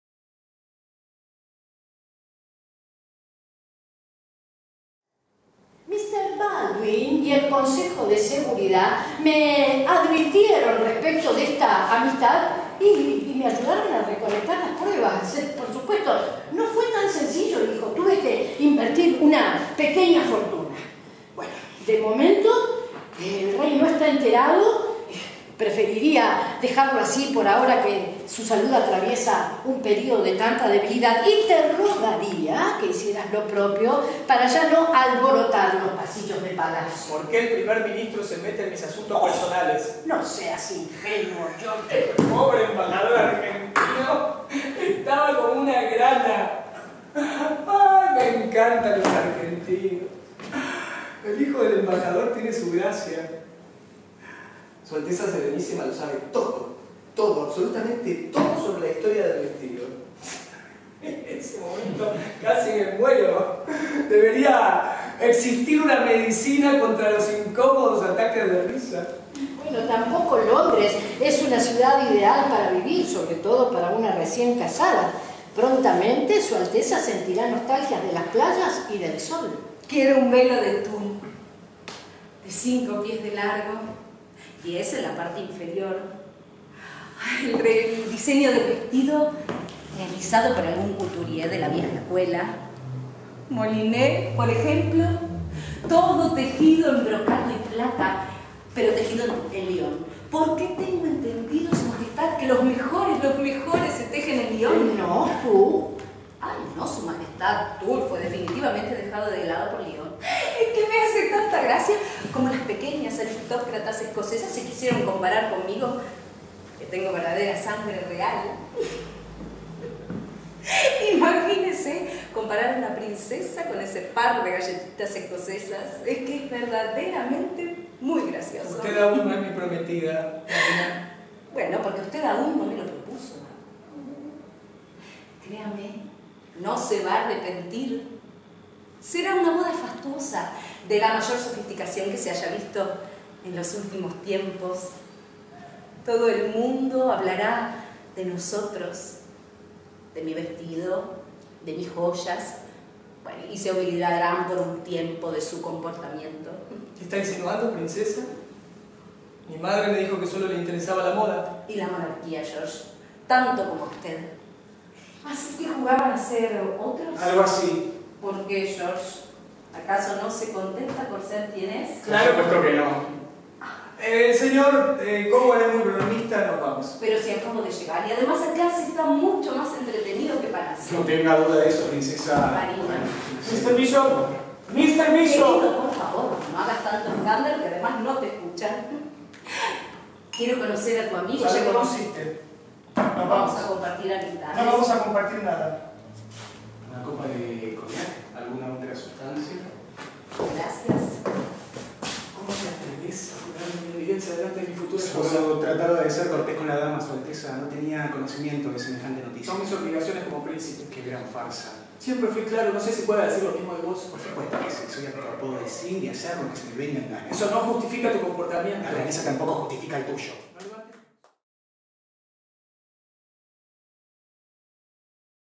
LA JOYA MÁS PRECIADA
Escenas de LA JOYA MÁS PRECIADA de Patricia Suárez y Sandra Franzen, con dirección de Graciela Pereyra. Actúan Alicia Muxo, Ulises Puiggrós, Débora Longobardi y Javier Báez. Teatro La Comedia. Abril 2017